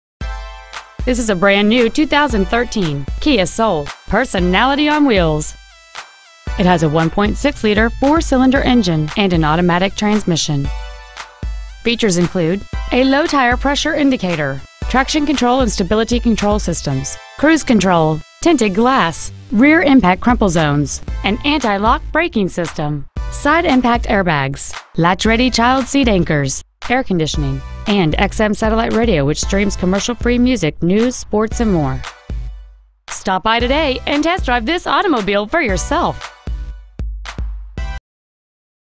2013 Kia Soul Auburn WA
Year : 2013

 Make : Kia

 Model : Soul

 Engine : 1.6L I4

 Trans . : Automatic

 Exterior : Molten Red

 Interior : Black

 111 Valley Ave NE

 2013 Kia Soul Puyallup WA